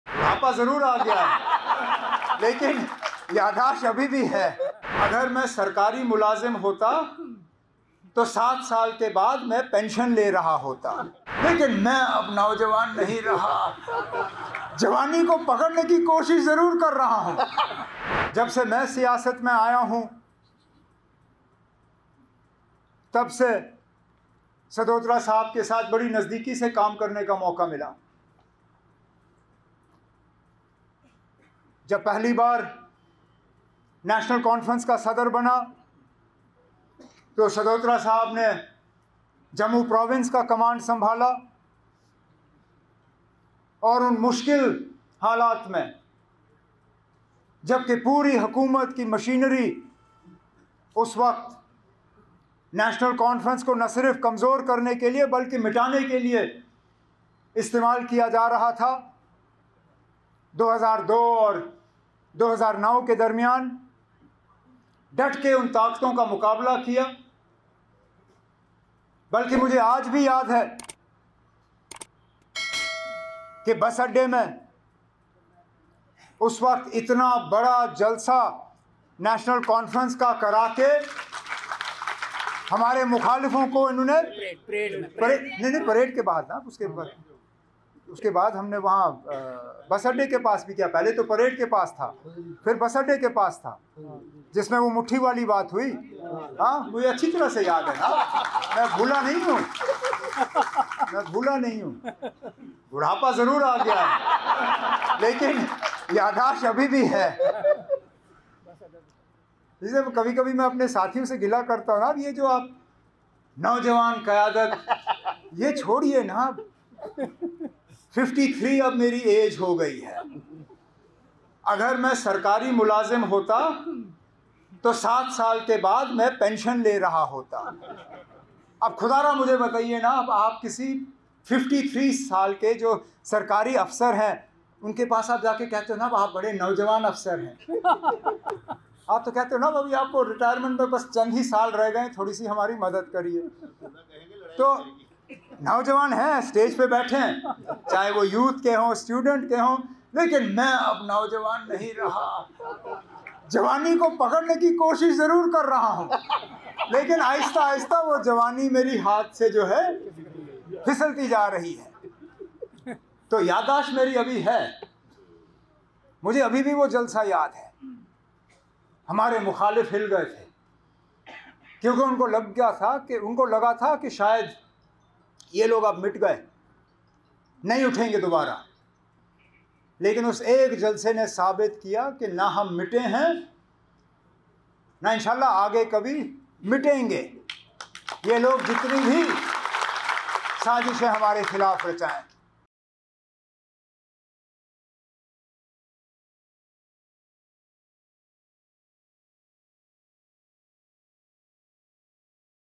National Conference के Vice-President Omar Abdullah ने क्यों बताया खुद को बूढ़ा?
नेशनल कॉन्फ़्रेंस(National Conference) के पार्टी ऑफिस में कार्यकर्ताओं के साथ हुई मीटिंग में उमर अब्दुल्ला(Omar Abdullah) ने कार्यकर्ताओं को सम्बोधित किया. उन्होंने पार्टी के सदस्यों की तारीफ की और साथ काम करने का अनुभव साझा किया. भाषण के दौरान ही उन्होंने कहा कि, "बुढ़ापा जरूर आ गया है लेकिन याददाश्त अभी भी सही है. 53 साल मेरी उमर हो गयी है. अगर मैं सरकारी मुलाज़िम होता तो 7 साल के बाद मैं पेंशन ले रहा होता."